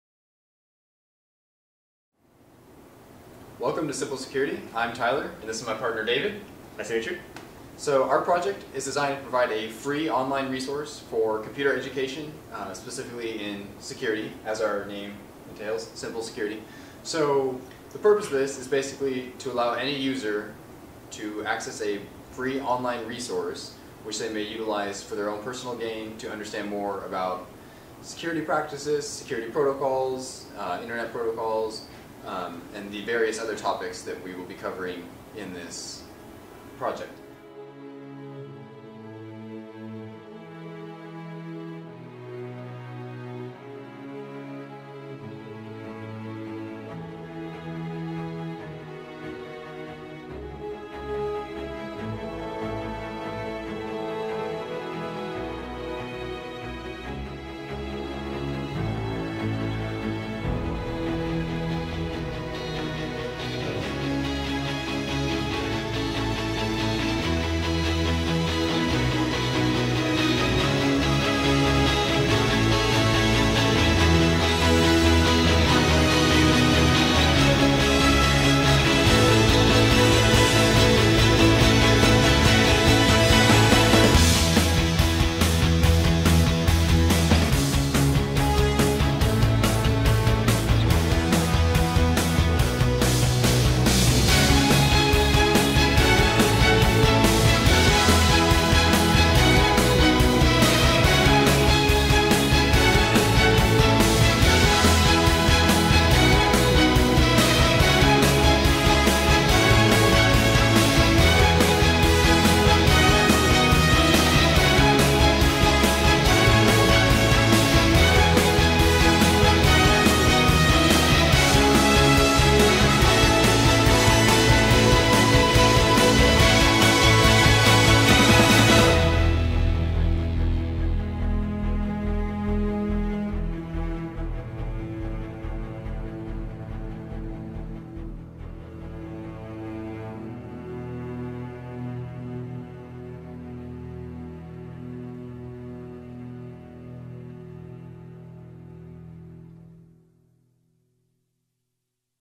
Simple Security Trailer
Welcome to Simple Security CBT! This free course provides an online tutorial covering the basics of Cyber Security. Topics include information assurance, social engineering, vulnerability analysis, online-privacy, personal computer security, network security, web security, common cyber attacks and their defenses, digital forensics, and risk management. This course is designed to give those with minimal technical experience the information needed to protect their digital presence, and their digital assets.

The Simple Security teaser trailer.